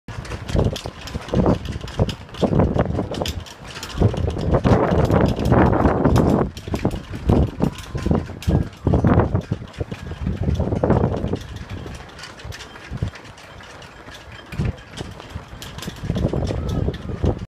Happy Flag Day - June 14, 2015 - Liberty State Park - Jersey City, New Jersey
Flags in the wind to celebrate Flag Day